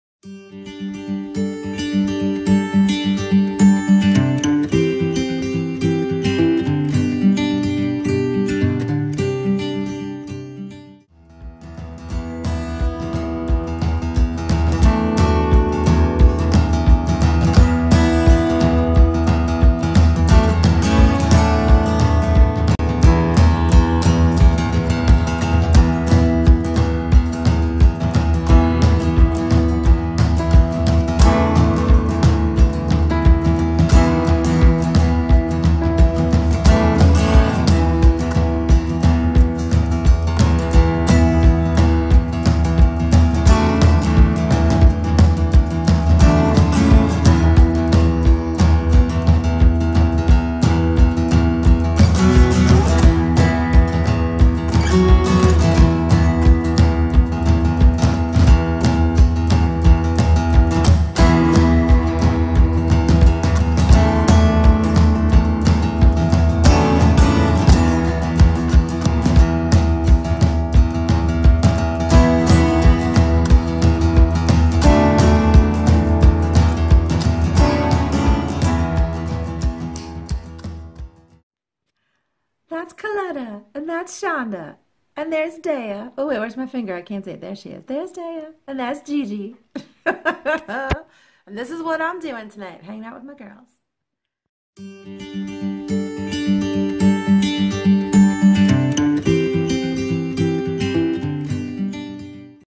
Vlogmas - Dec. 14th
Today's video is not my best.  I'm slammed at the shop and my brain is over-filled with things to remember that taking video didn't fall on the radar today.  But I guess it's to be expected along the way as I document my Vlogmas 2017.